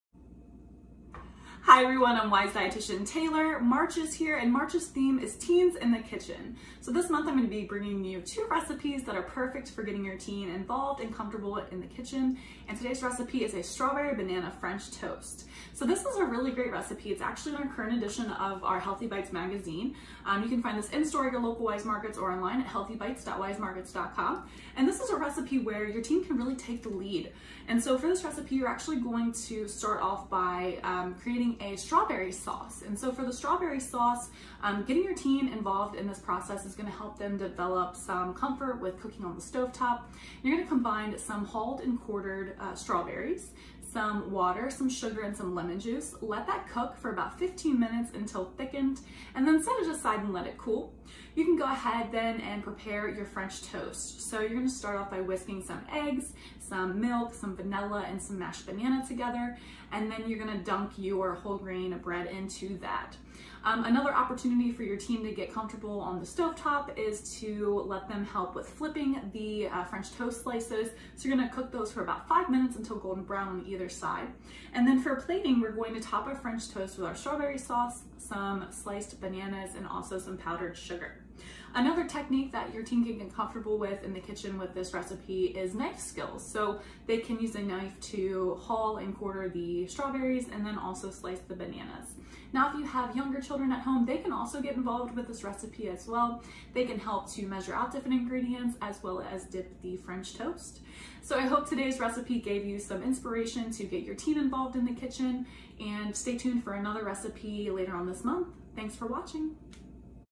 Strawberry Banana French Toast
Strawberry Banana French Toast by Taylor Simpson, MS, RDN, LDN - In-Store Registered Dietician Nutritionist at Weis Markets.